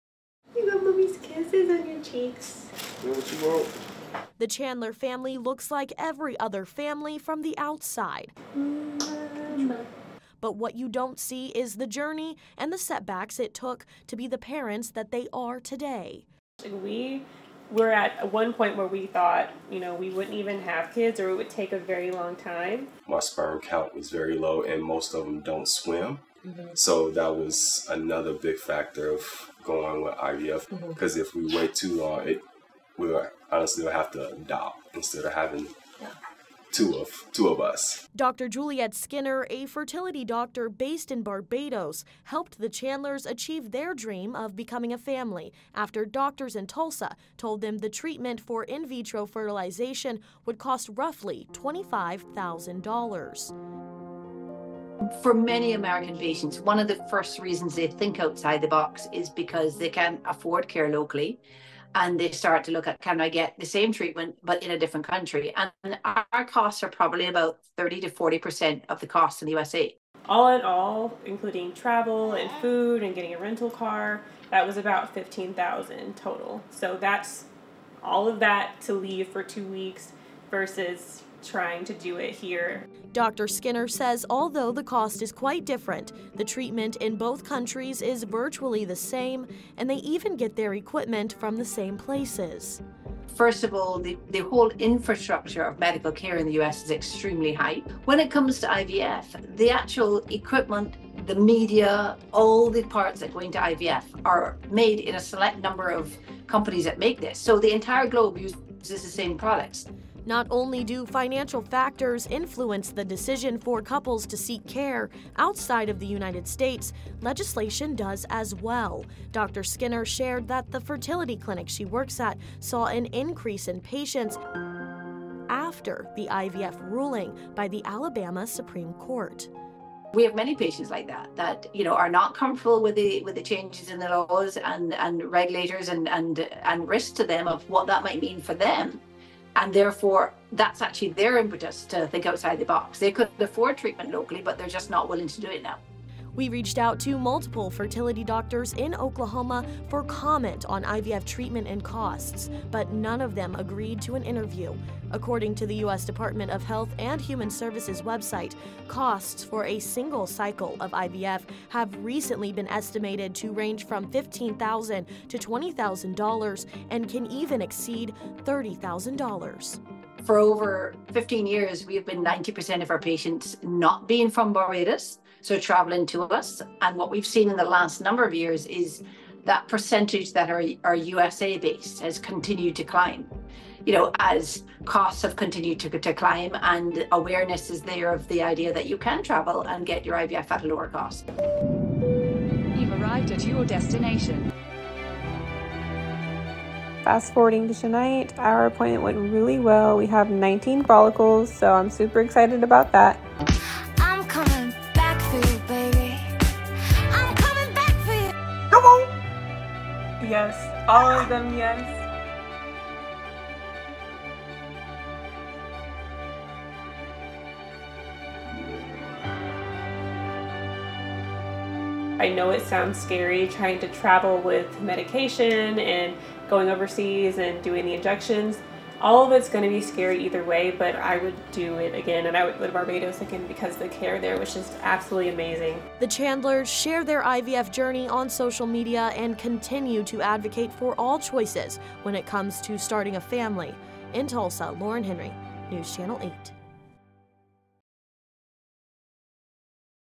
Going to Barbados for IVF Treatment
2024 news story about Barbados becoming a more and more popular destinations for those seeking IVF.